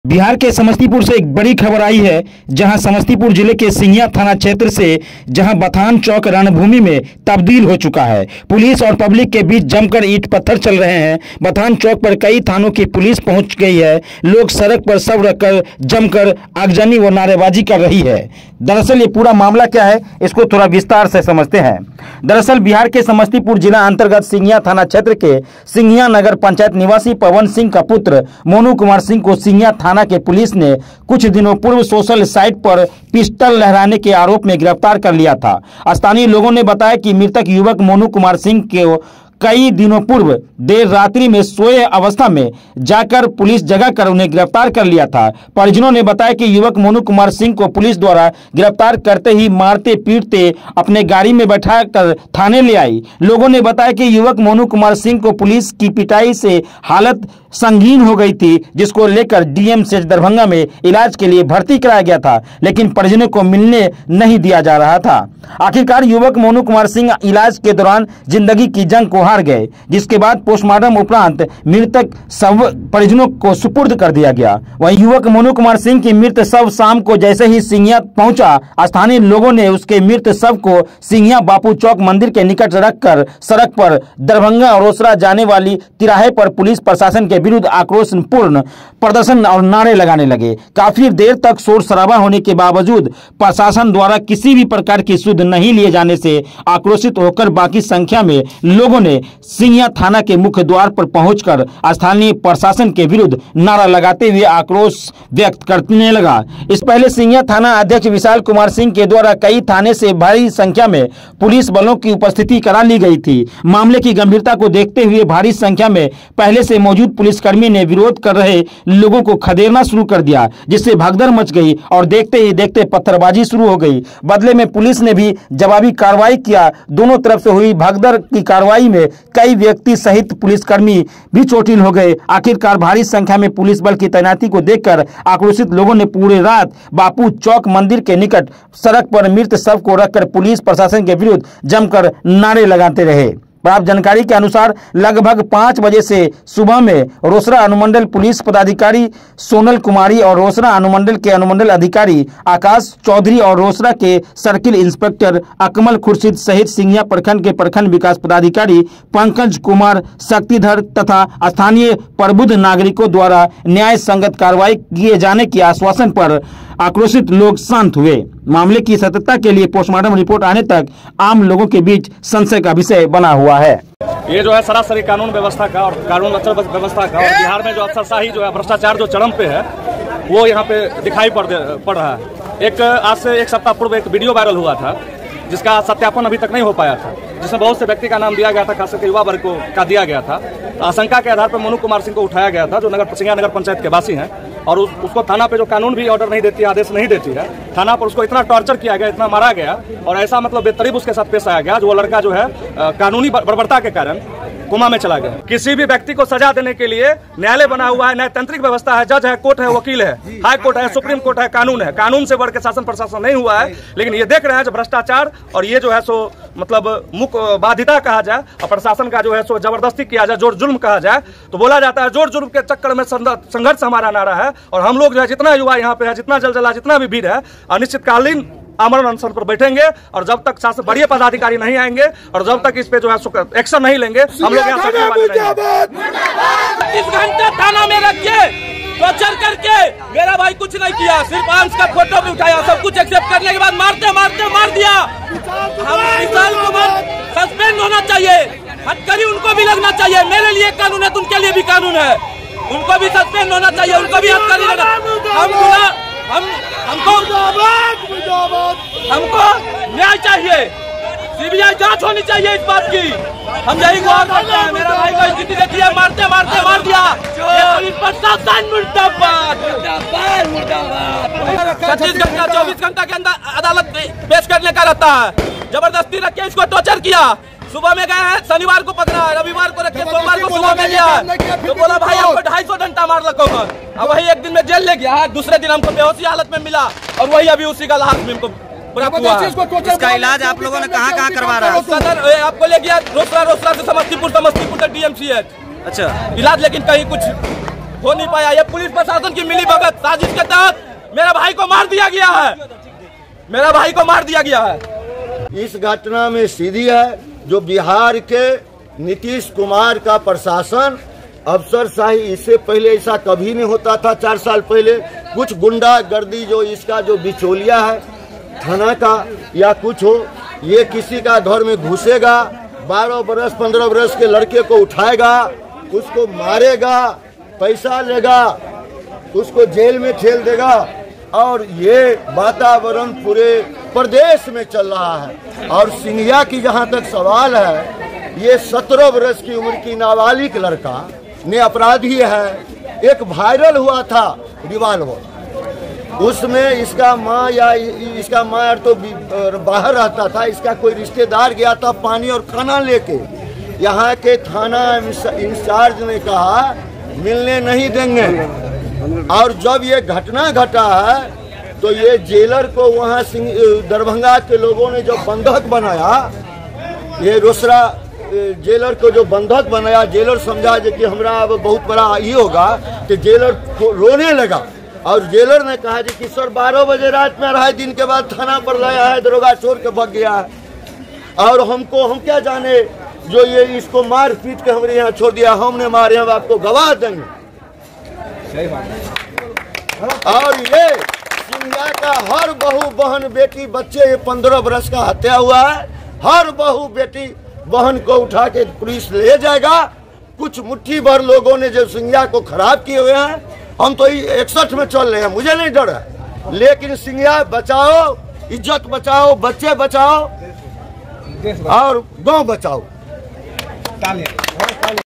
पुलिस अभिरक्षा में इलाजरत सिंघिया के मोनू सिंह ,मुख्य सड़क को जाम कर दरोगा को बर्खास्त
पुलिस अभिरक्षा में इलाजरत सिंघिया के मोनू सिंह ,मुख्य सड़क को जाम कर दरोगा को बर्खास्त की मांग